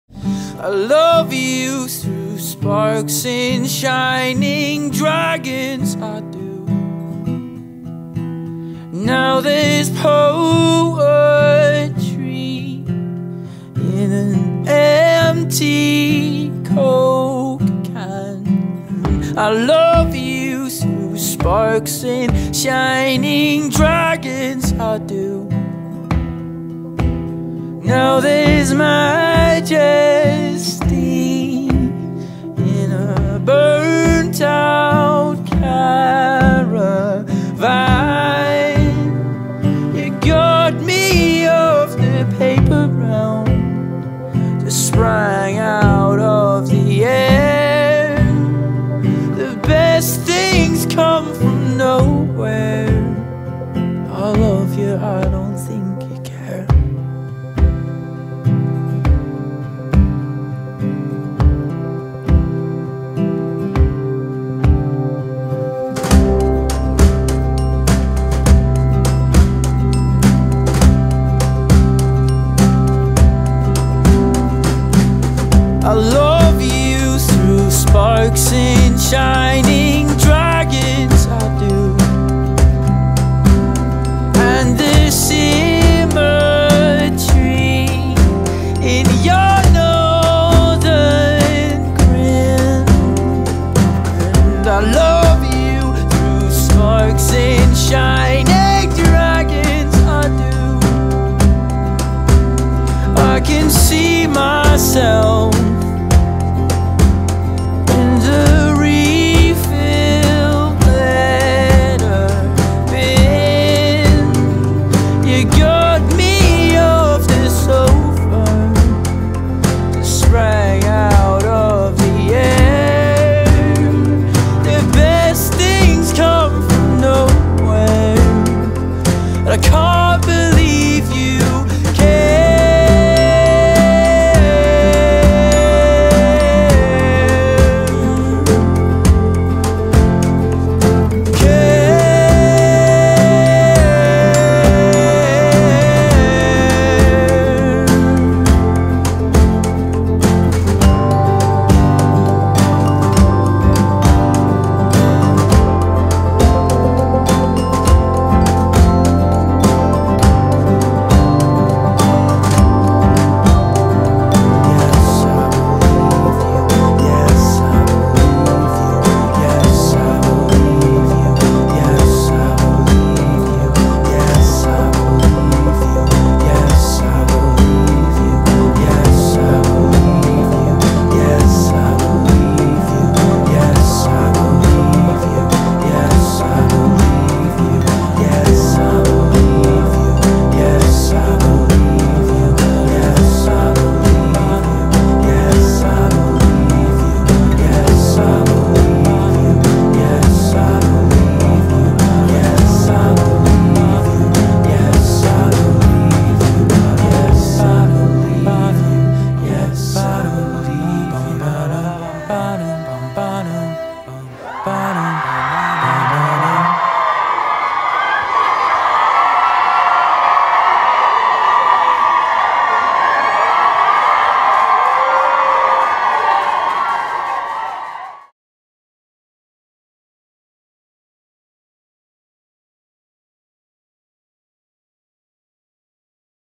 lewis watson - because the last one was so great tour
here's some footage from my most recent tour 'because the last one was so great'. thanks so much for coming, it was a ball and i can't wait to get back on the road.